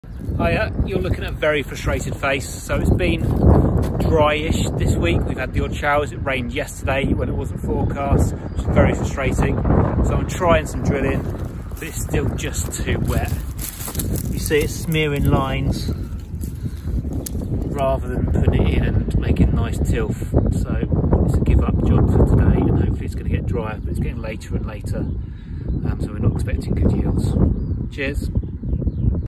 Still too wet to plant this spring wheat
Very frustrating day on Saturday. I tried sowing our spring wheat with the Vaderstad seed drill, but it was still way too wet. 🤞the weather gets dry and we can try again soon!